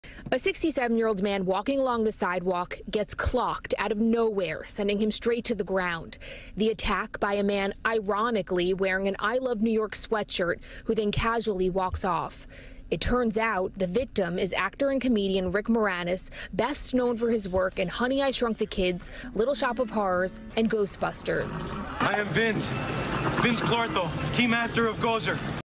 Actor Rick Moranis ATTACKED CAUGHT CAMERA...
Actor Rick Moranis was attacked near his home on the Upper East Side of New York city and the attack was caught on camera. The suspect is still at large.